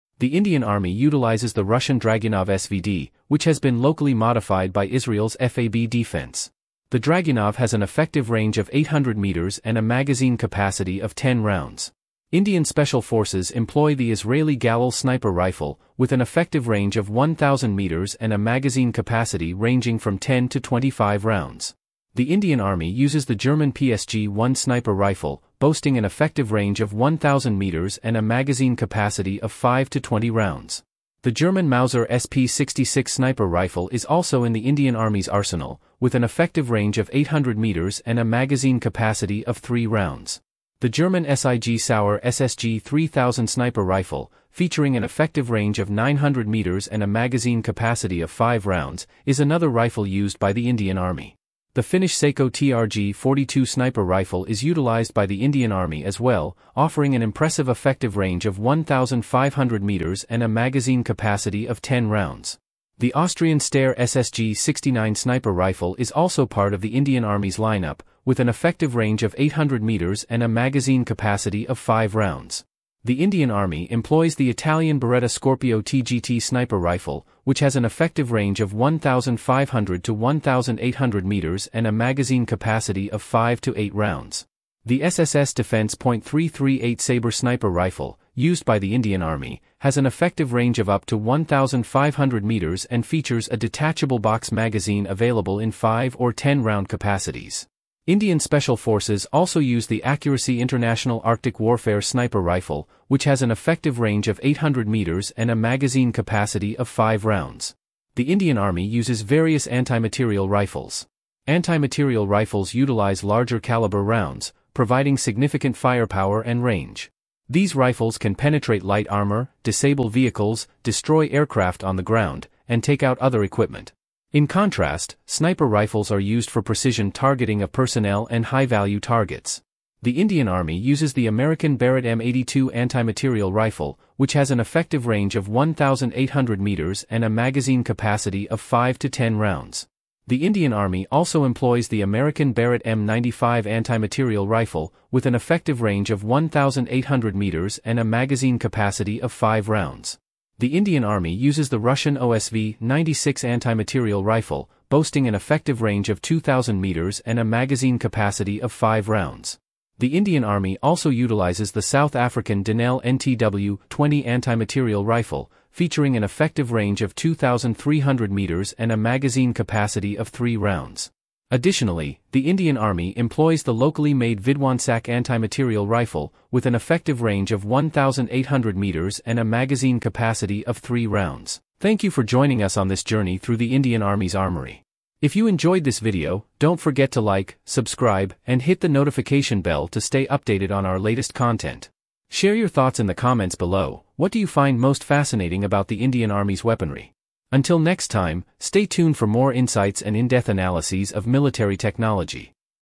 Indian Army Sniper Rifles | Indian Army Anti Material Rifles | Indian Army Guns 2024
Indian Army Sniper Rifles | Indian Army Anti Material Rifles | Indian Military Weapons 2024 | SaqibZiaTV

In This video I will tell you about the sniper rifles and anti material rifles currently used by Indian Army or Indian Military.

A sniper rifle is a high-precision, long-range rifle. Requirements include high accuracy, reliability, and mobility, concealment, and optics, for anti-personnel, anti-materiel and surveillance uses by military snipers. The modern sniper rifle is a portable shoulder-fired rifle with either a bolt action or semi-automatic action, fitted with a telescopic sight for extreme accuracy and chambered for a high-ballistic performance centerfire cartridge.

An anti-materiel rifle (AMR) is a rifle designed for use against military equipment, structures, and other hardware (materiel) targets. Anti-materiel rifles are chambered in significantly larger calibers than conventional rifles and are employed to eliminate equipment such as engines and unarmored or lightly armored targets. Although not originally designed for use against human targets, the bullet weight and velocity of anti-materiel rifles gives them exceptional long-range capability even when compared with designated sniper rifles. Anti-materiel rifles are made in both bolt-action as well as semi-automatic designs.

The anti-materiel rifle originated in the anti-tank rifles.While modern tanks and most other armored vehicles are too well protected to be affected by anti-materiel rifles, the guns are still effective for attacking unarmored or lightly armored vehicles. They can also be used against stationary enemy aircraft, missile launchers, radar equipment, unexploded ordnance, small watercraft, communications equipment, crew-served weapons and similar targets. Their value lies in their ability to precisely target and disable enemy assets from long range at relatively low cost